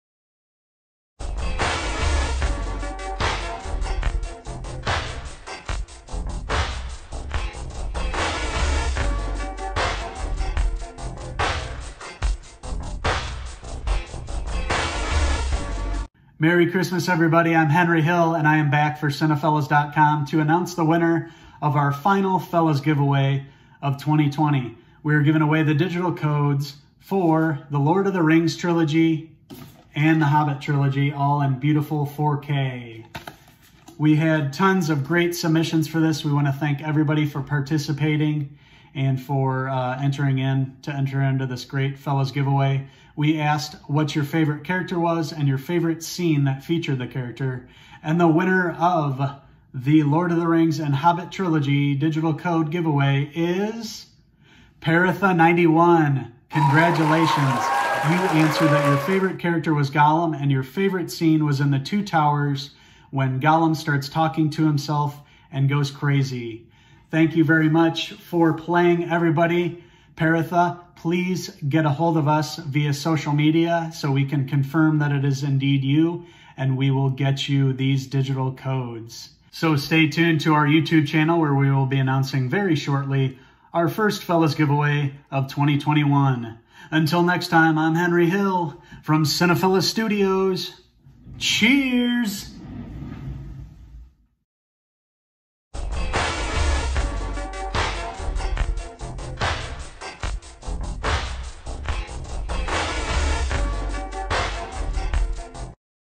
LOTR and The Hobbit Trilogy - Digital Code PhellasGiveAway Winner!
We wanted to congratulate the winner of the Lord of The Rings and The Hobbit digital code PhellasGiveAway ! We'll be announcing another PhellasGiveAway later this week for the first giveaway of 2021.